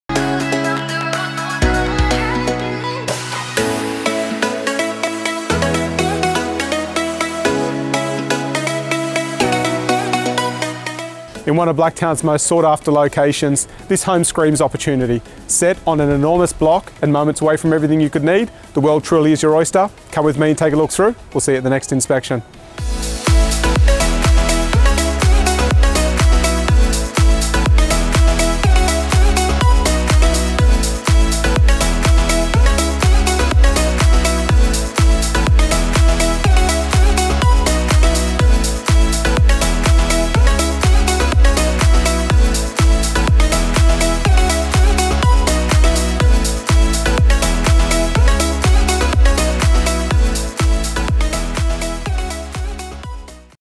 29 Lander Avenue, Blacktown 2148
Homes such as this are seldom found yet highly sought-after. Located in arguably the most sought-after street in Blacktown - finding a better opportunity to build your dream home is almost impossible. Take advantage of the endless opportunities presented with the enormous block with its wide frontage. Whether you're looking to knock-down and rebuild, rent out as-is or add a granny flat for a great rental yield, the choice is yours. Wait too long and miss out on this once in a lifetime property.
The home offers:
- 695.6m2 block with 15.24m frontage (approx)
- 2 large bedrooms
- Recently renovated throughout
- Modern kitchen and bathroom
- Detached lock-up garage
- Located under 1km from Blacktown Railway Station